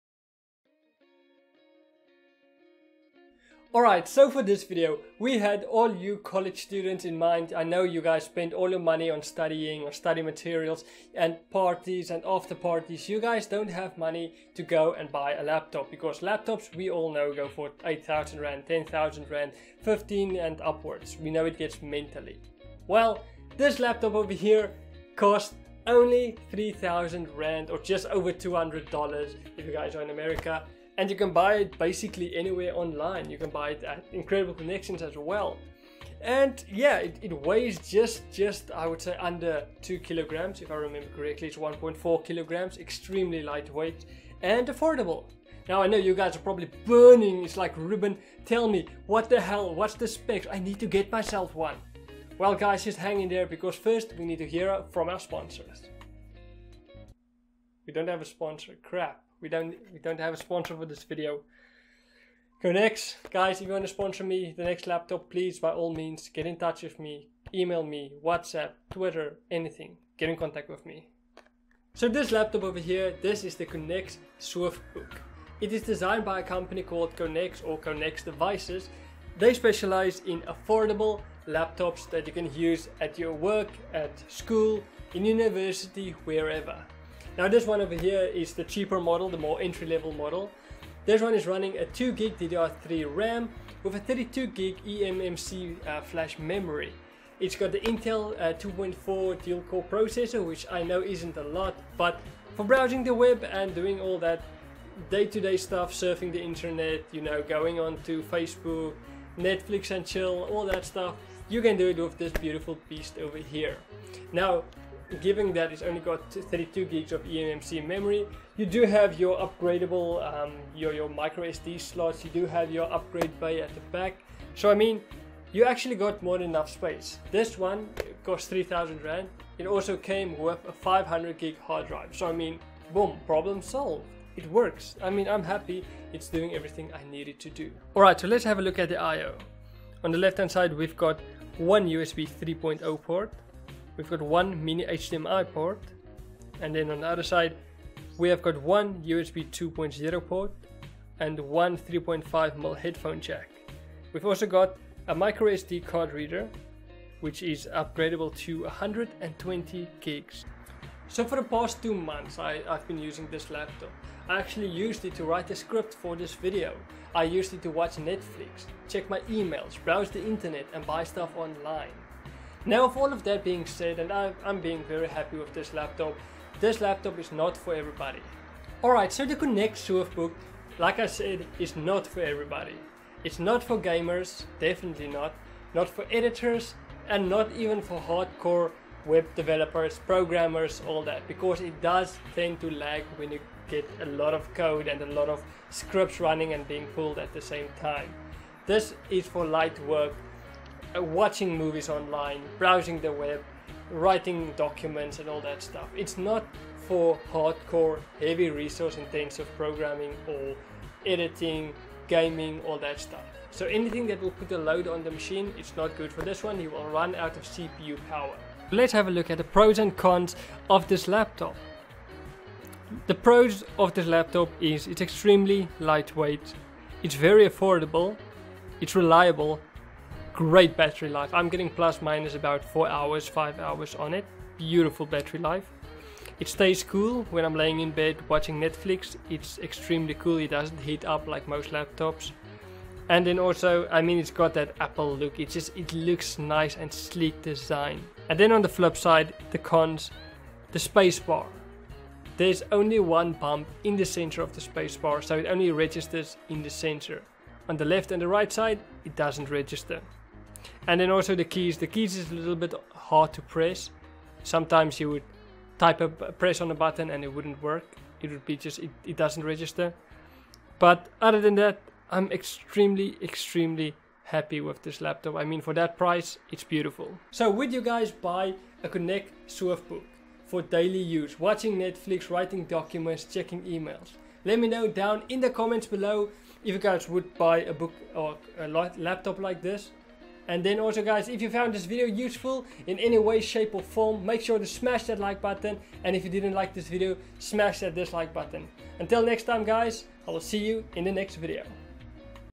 Connex Swift Book Review | Tech Land Media
If you are looking for a affordable laptop that you can use in your daily life then watch this video to the end.